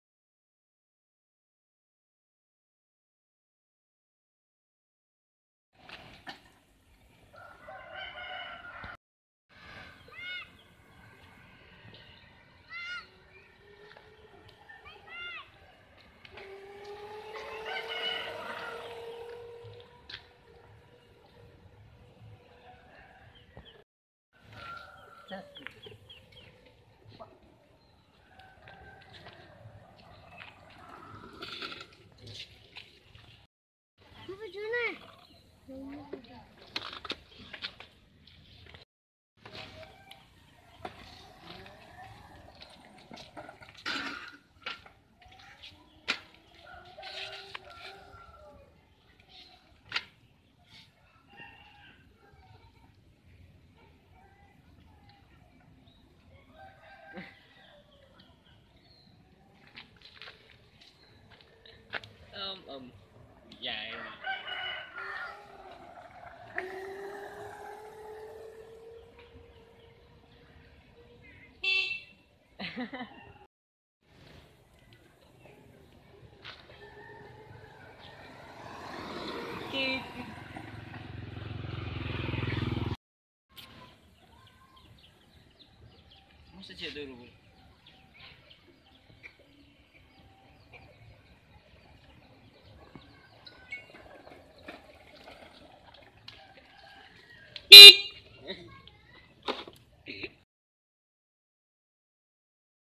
test drive scooter listrik Mr.Jackie untuk Anak
test drive scooter Mr.Jackie untuk anak SD. Cukup kencang dgn kecepatan kisaran 30-50km/jam. cocok untuk bermain anak